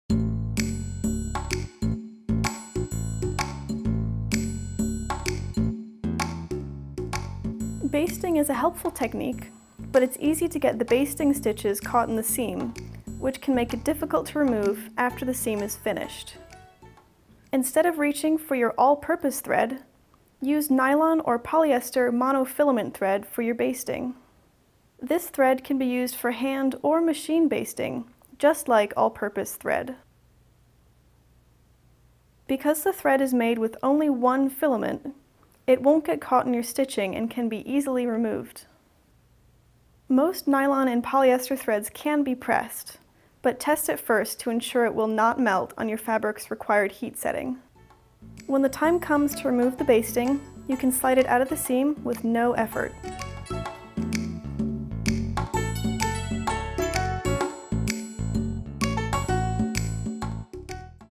Baste with Monofilament Thread
Monofilament thread has many uses, but you might not have considered its benefits as a basting thread.